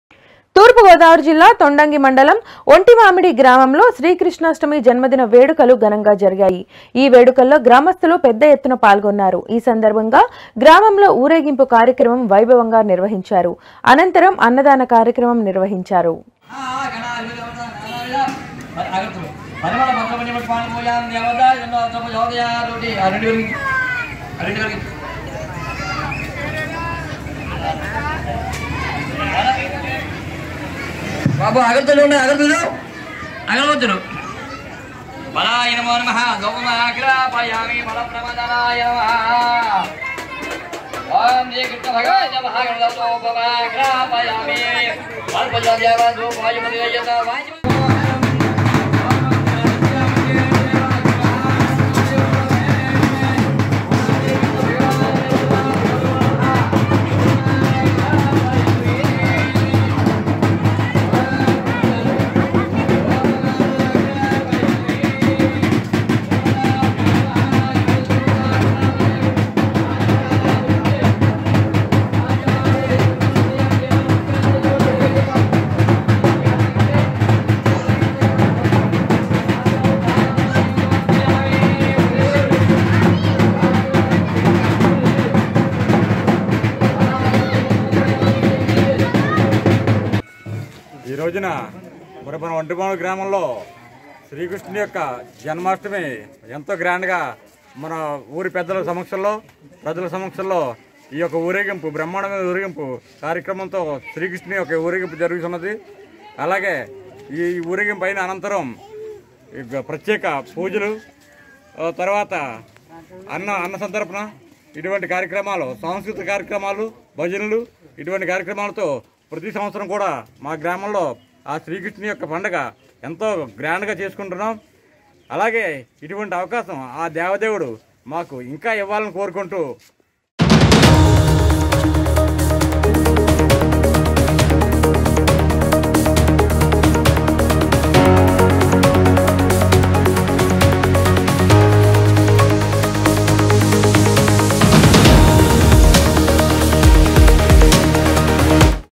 Krishnashtami birthday celebrations in Ontimamidi village | g9tv
తూర్పు గోదావరి జిల్లా తొండంగి మండలం ఒంటిమామిడి గ్రామంలో శ్రీకృష్ణాష్టమి జన్మదిన వేడుకలు ఘనంగా జరిగాయి. వేడుకల్లో గ్రామస్థులు పెద్ద ఎత్తున పాల్గొన్నారు. ఈ సందర్భంగా గ్రామంలో ఊరేగింపు కార్యక్రమం వైభవంగా నిర్వహించారు. అనంతరం అన్నదాన కార్యక్రమం నిర్వహించారు.